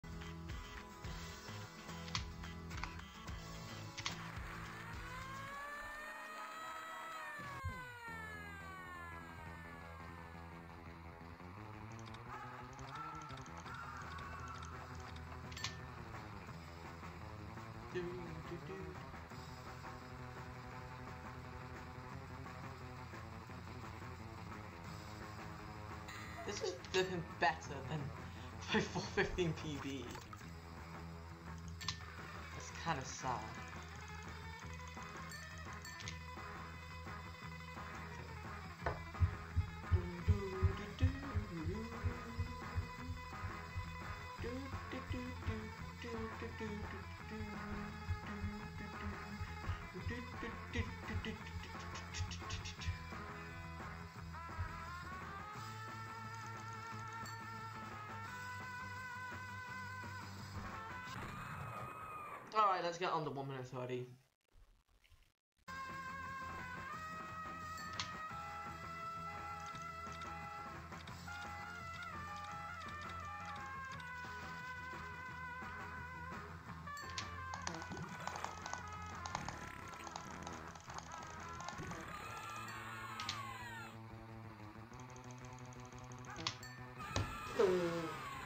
The epic 10 in 1;22.430
i saw this new catergory and wanted to get 1st place before pianodud gets a stupid time so i can take a photo etc...